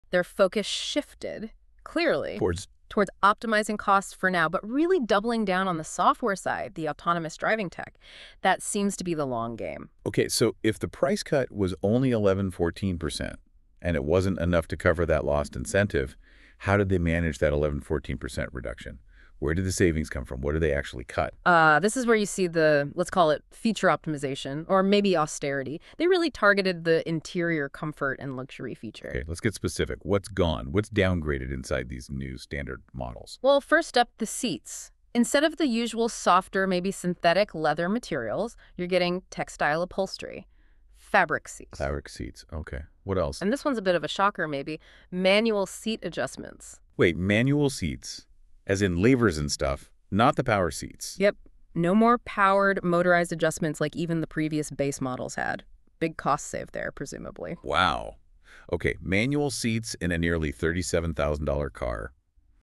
How Did Tesla Make Their Cars Cheaper?
Austerity Measures at Tesla? This video examines how **tesla price** reductions were achieved through **feature selection** and other **economics** driven initiatives. The focus seems to be shifting towards **tesla fsd** and software, so the **tesla** is cutting back on some features to lower the prices of their **tesla cars**.